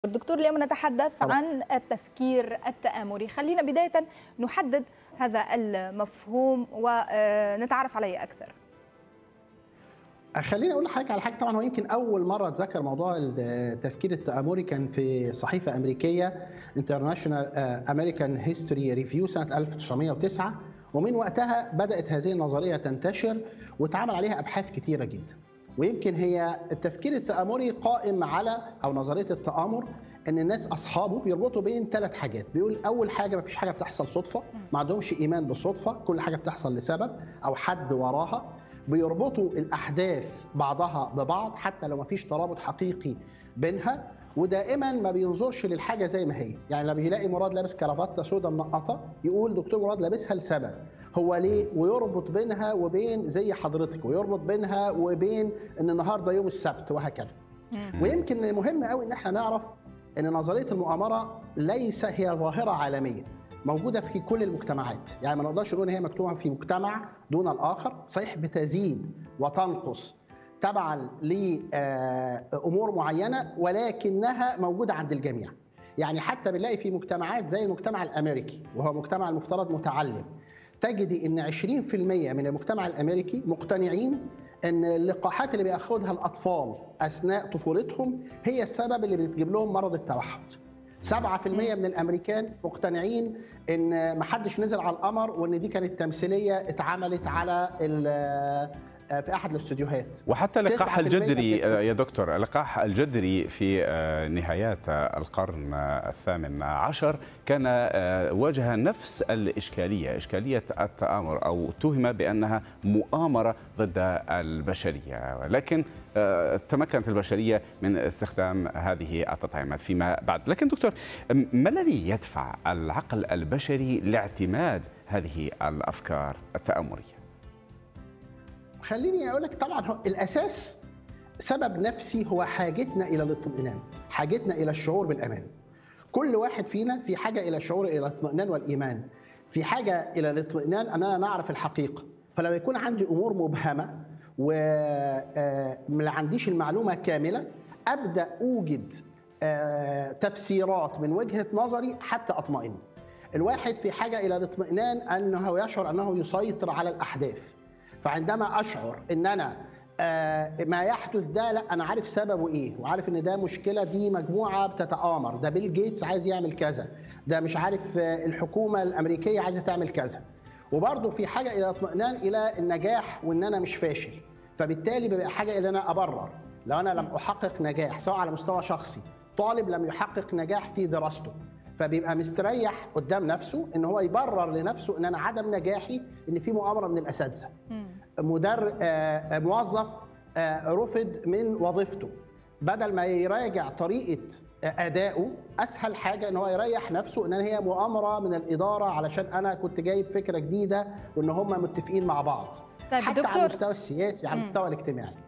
حوار تلفزيوني: ما حقيقة نظرة المؤامرة؟ أسباب التفكير التآمري
في أمريكا ٢٠٪ مقتنعين أن تلقيح الأطفال في صغرهم ضد شلل الأطفال والجدري  هو سبب إصابتهم بالتوحد و٤٦٪ فقط هم المقتنعين
بأمان هذه اللقاحات
سبعة في المائة من الأمريكان مقتنعين إن الإنسان لم ينزل علي القمر وإن اللي اتنشر ده كان تمثيلية اتعملت في استوديو و٩٪ غير متأكدين
تسعة في المائة مؤمنين إن حكومة الولايات المتحدة تضيف الفلورايد للماء ليس لتقوية الأسنان ولكن للسيطرة عليهم و١٧٪ لا يثقوا في هذا الإجراء
في حوار مع قناة الجزيرة، أستعرض حقيقة نظرية المؤامرة، تاريخها ولماذا يلجأ البعض إليها؟